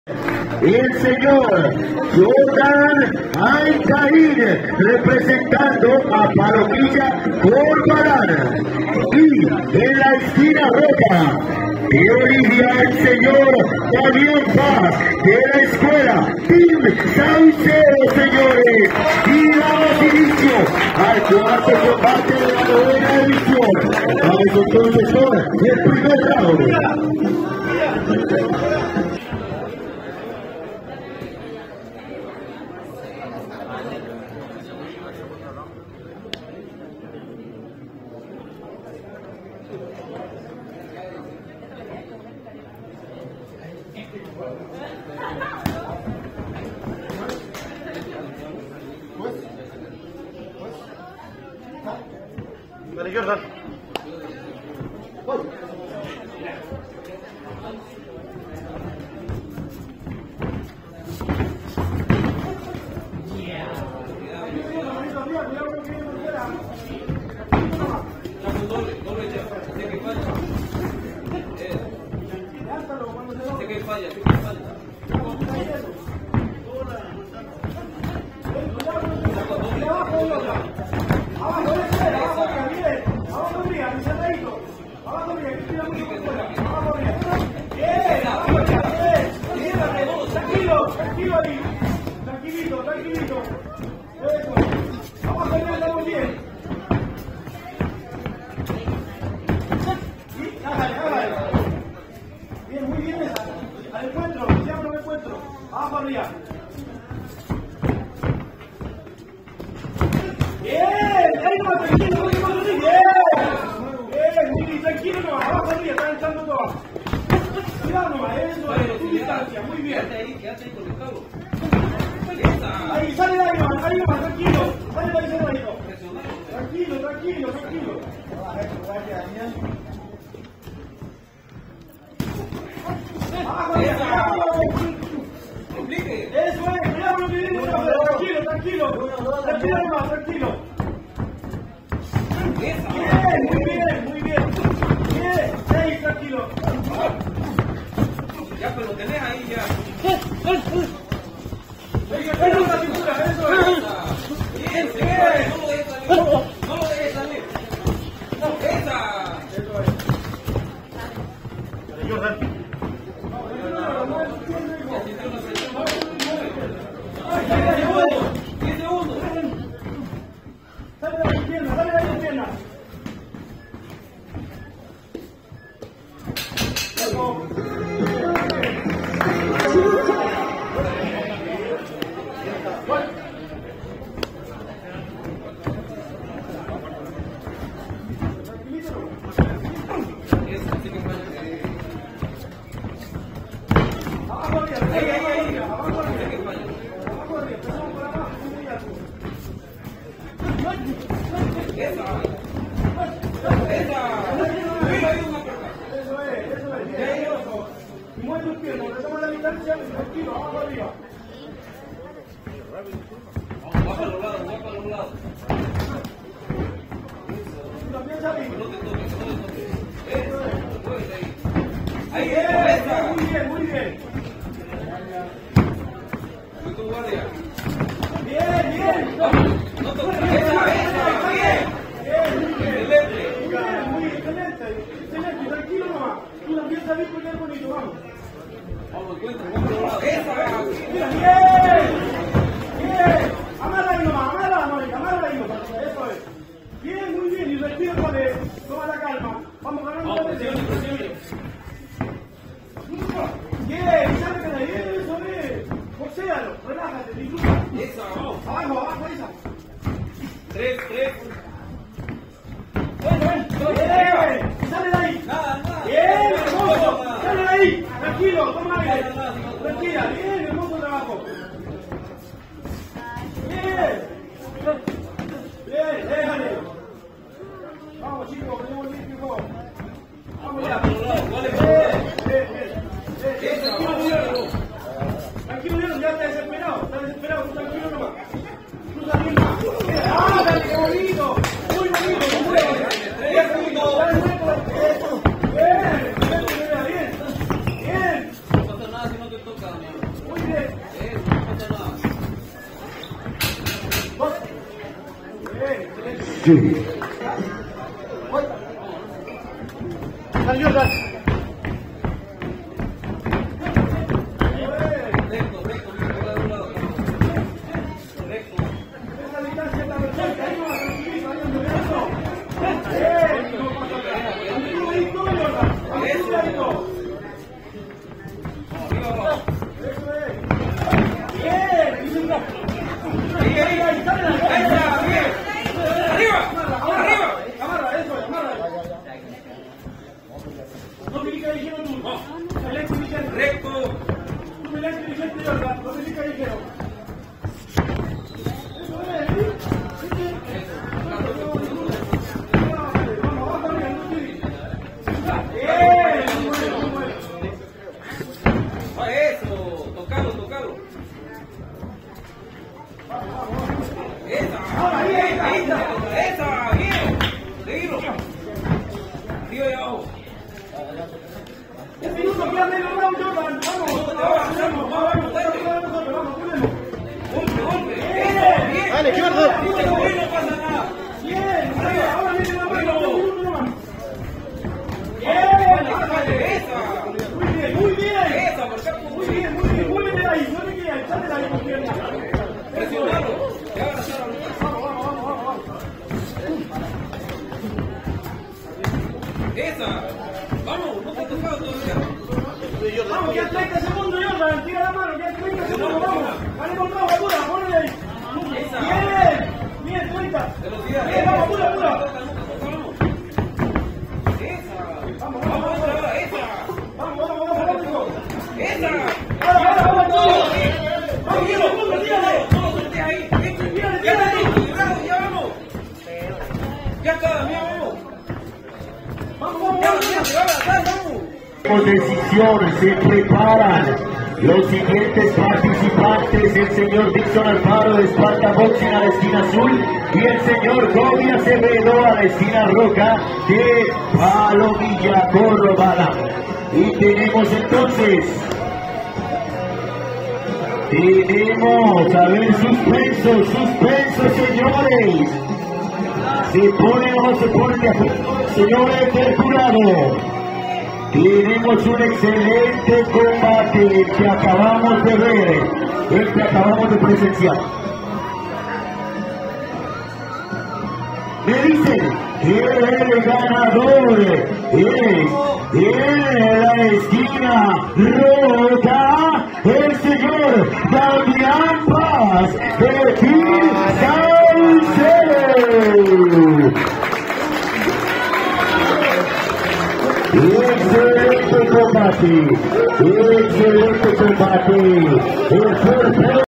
Jordan Ancain (esquina roja) vs Damián Paz (esquina azul)
COLLISION 9. Combate juvenil internacional. Desde Bolivia nos visitó el Team Saucedo y se enfrentaron a La Fundación Palomilla Corvalan